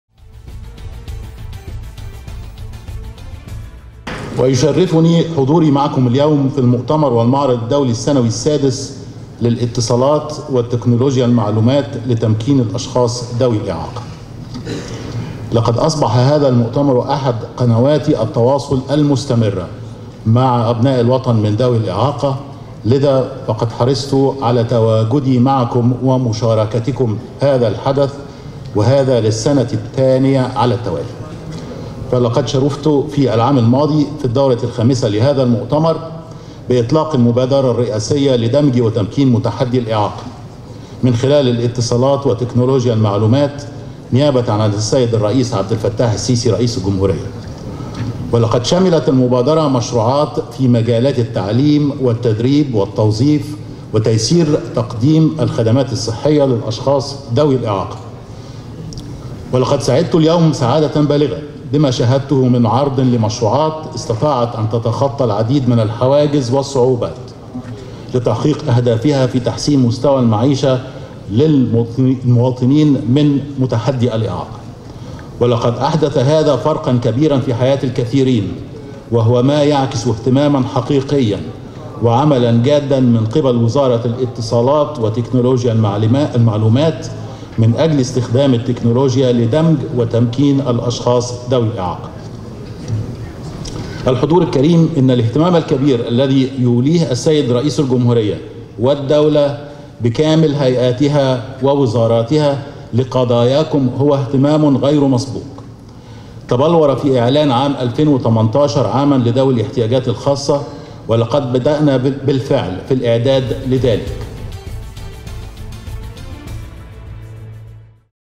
مصر العربية | رئيس الوزراء: اهتمام الدولة بقضايا ذوى الإعاقة غير مسبوق
قال المهندس شريف إسماعيل، رئيس الوزراء ، فى كلمته بمؤتمر وزارة الاتصالات لتمكين الأشخاص ذوى الإعاقة، أن الاهتمام الذى يوليه رئيس الجمهورية والدولة لقضايا ذوى الإعاقة غير مسبوق، ويتم حاليا الإعداد لأن يكون عام ٢٠١٨ هو عام ذوى الإعاقة .